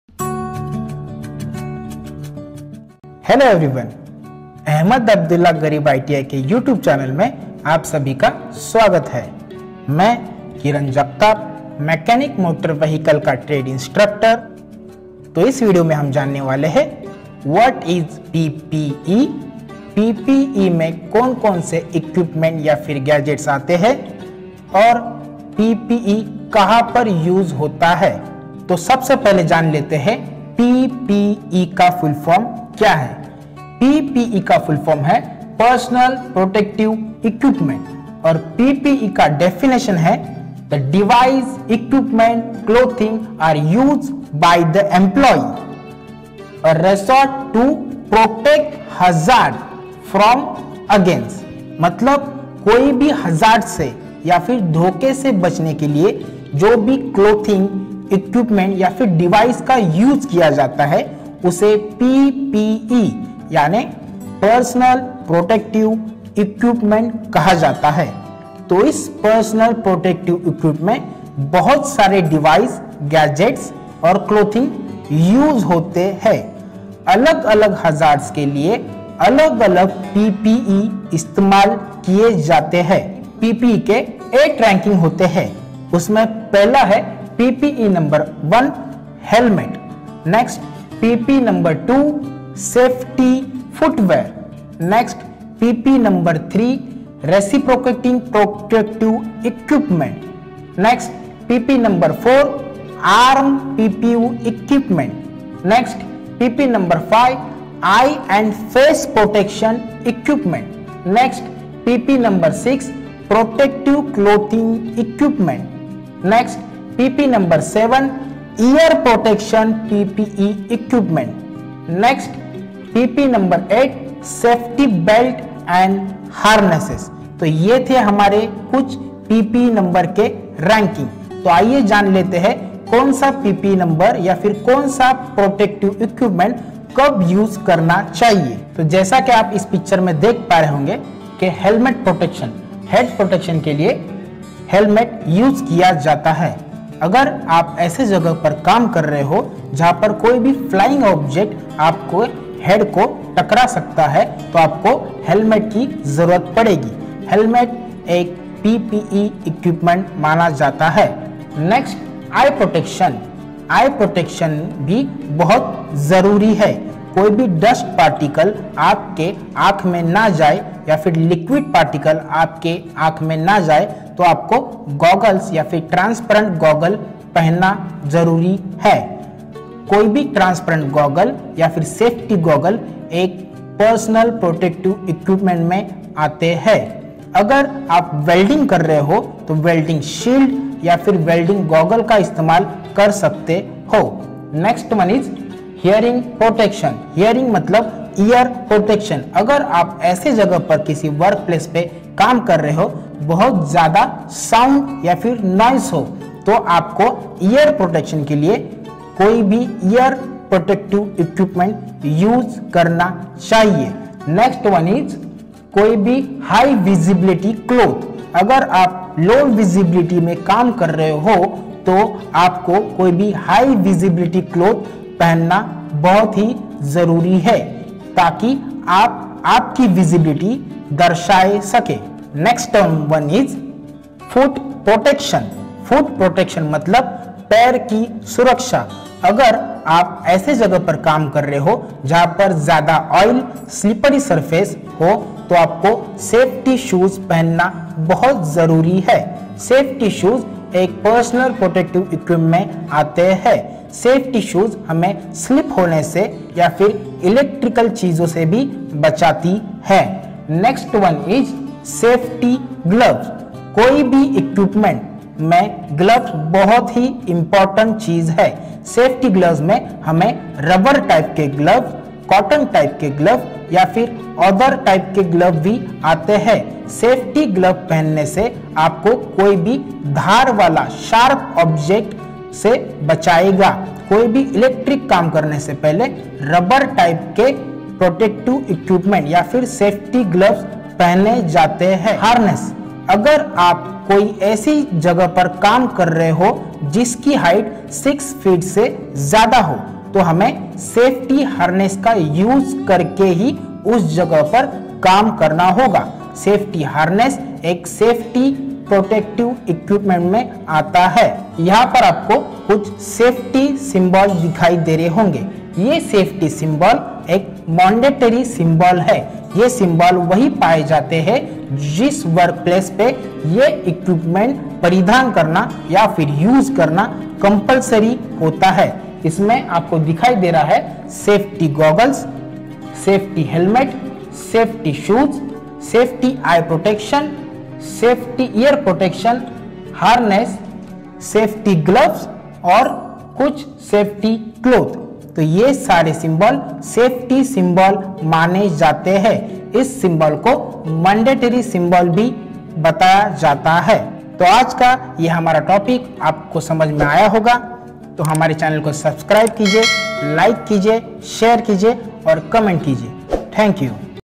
DETAIL ABOUT P.P.E. (TRADE MMV)
In this video we will learn about "P.P.E." Presented by Mr. Kiran Jagtap - Mechanic Motor Vehicle Trade Instructor from Ahmed Abdullah Garib P.I.T.I.. We hope it will be useful for all students (Trainees).